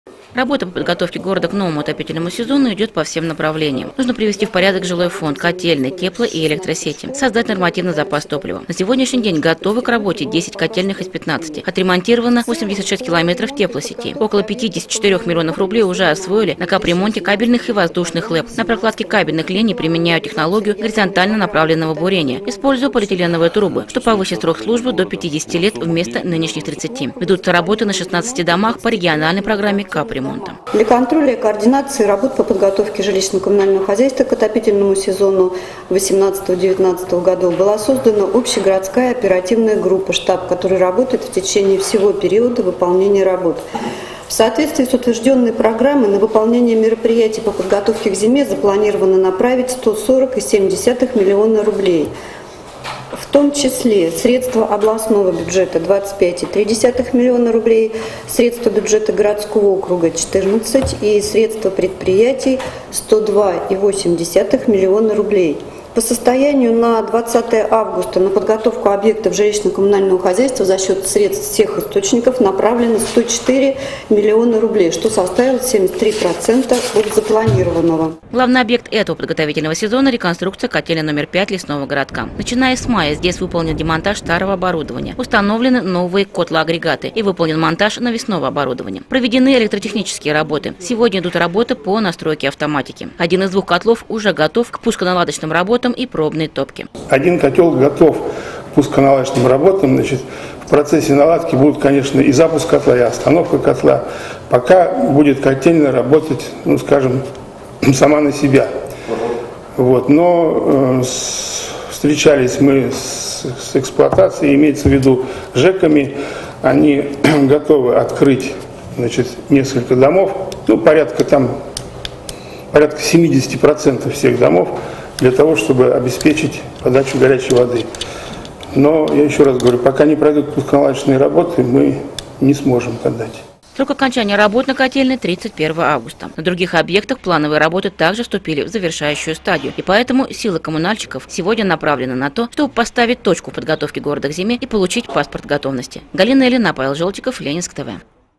Город готовится к зиме
Ход подготовки объектов ЖКХ Ленинска-Кузнецкого к зиме стал темой заседания коллегии администрации, которое провёл глава городского округа Константин Тихонов. В общей сложности, на подготовку города к новому отопительному сезону планируется направить свыше 140 миллионов рублей, в том числе 28 миллионов по программе «Энергосбережение и повышение энергетической эффективности». На сегодняшний день порядка 80 % из запланированного объема уже сделано.